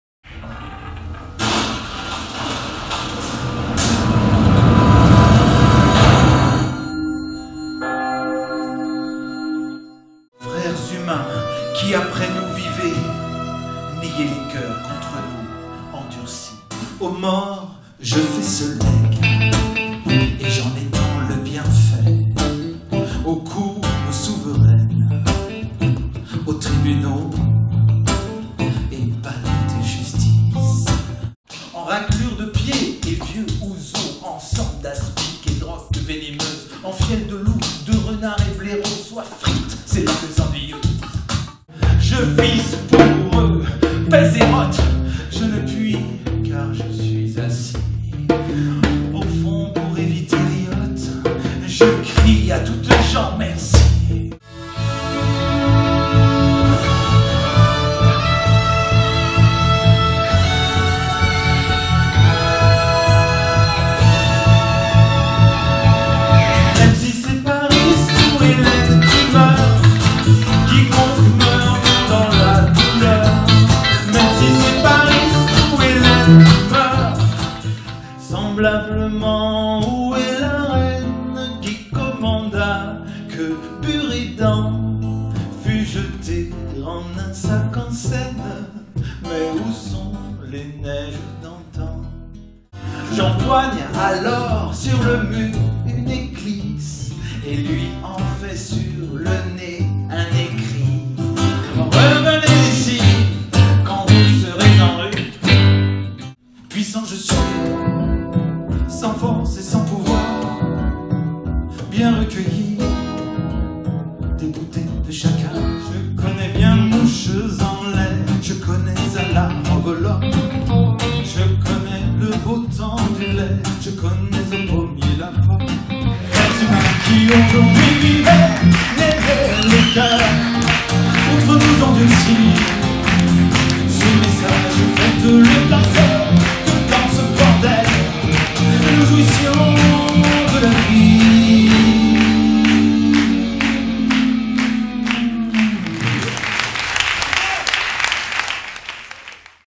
François Villon chanté par Jean Bruno Chantraine
Teaser du spectacle de Jean-Bruno Chantraine, sur des idées de Jean-Louis Lascoux : Villon, le coupable d'idéal.